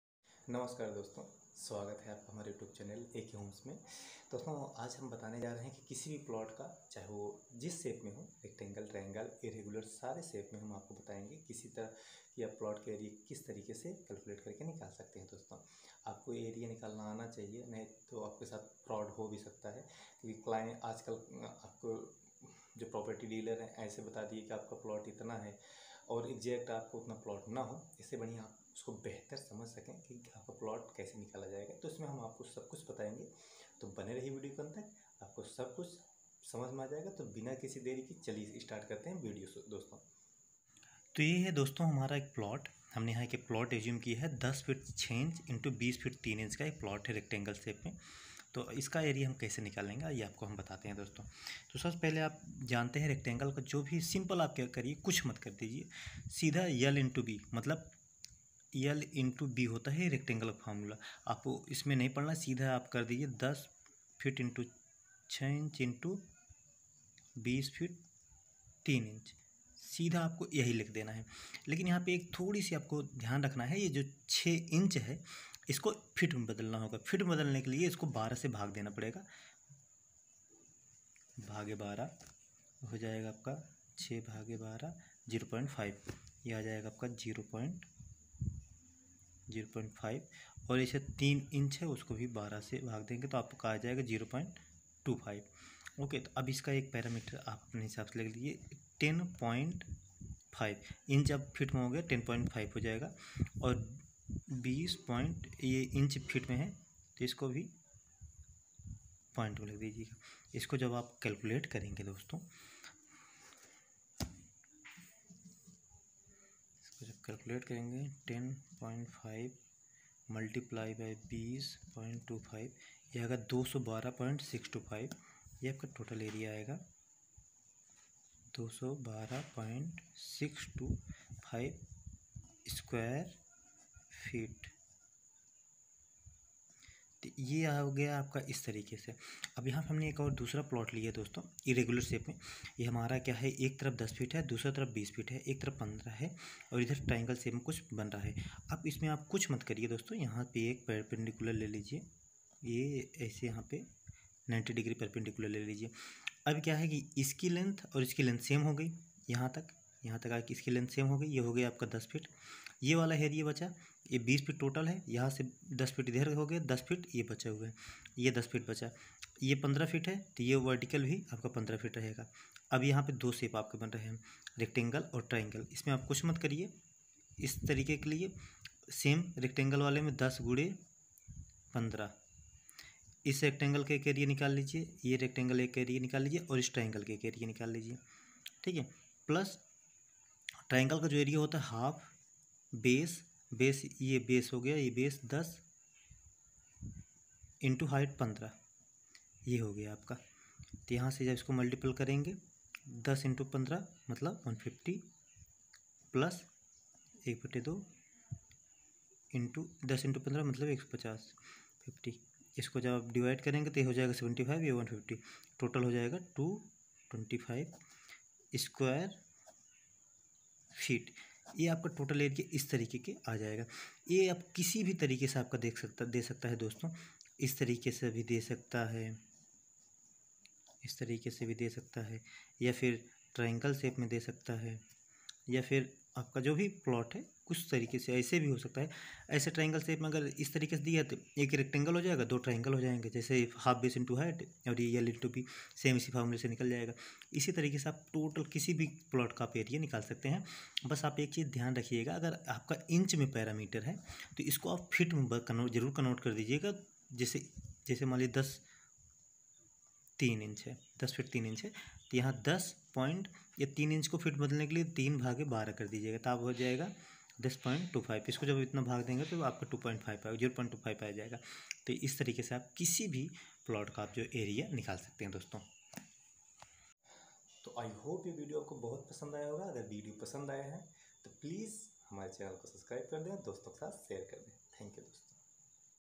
कोई भी Plot का क्षेत्रफल कैसे ज्ञात करें। How to Find plot area of any Plot
hello viewer
Welcome in my channel aaj ke video me hm apko batane ja rhe h ki kisi bhi plot ka area kaise nikala jata hai.
Ummeed karta hu ye video apko bahot pasand aya hoga.